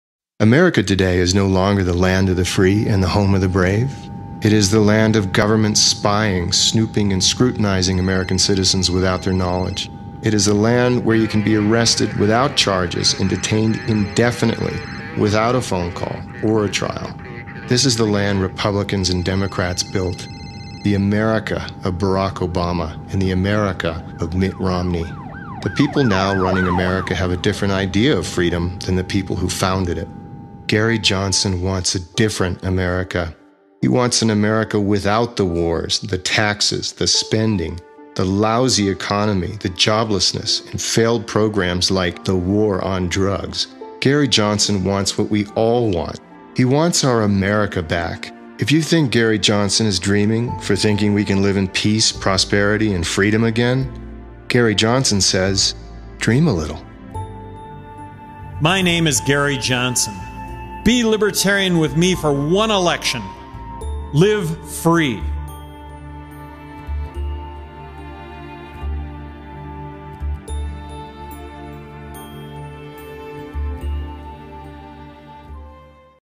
GARY JOHNSON Dream A Little
Gary Johnson challenges Americans to live the dream of peace and proposerity.